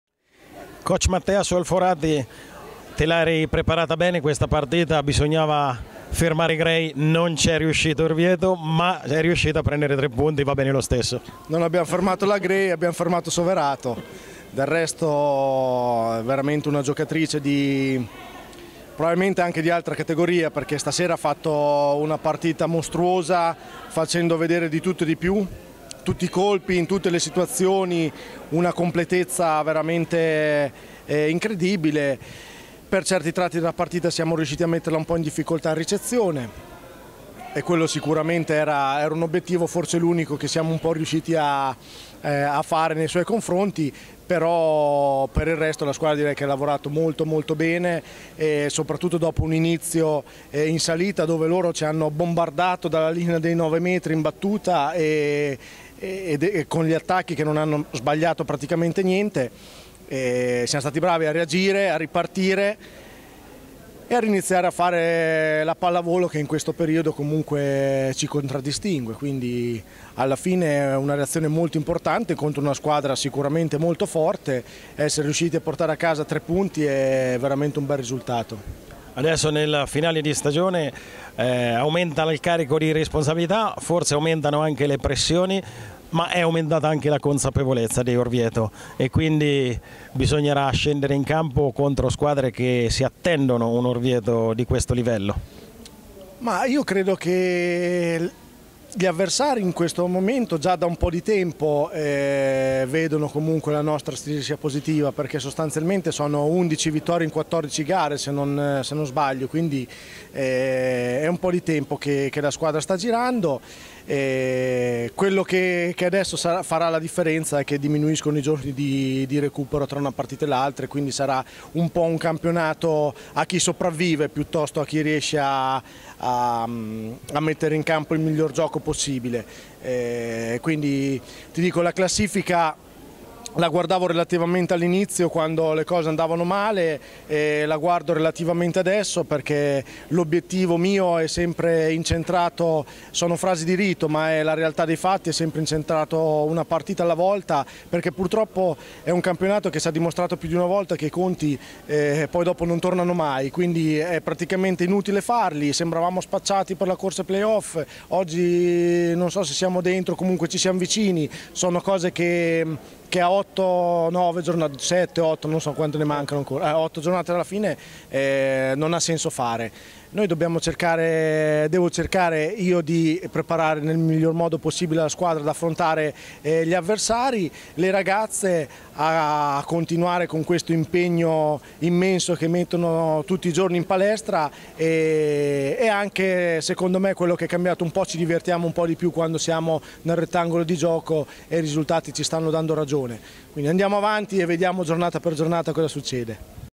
Solforati Matteo (Orvieto-Soverato)
intervista a Matteo Solforati allenatore della Zambelli Orvieto di serie A2 volley femminile stagione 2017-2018 (dopo match Ovieto-Soverato 27ª giornata di campionato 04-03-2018)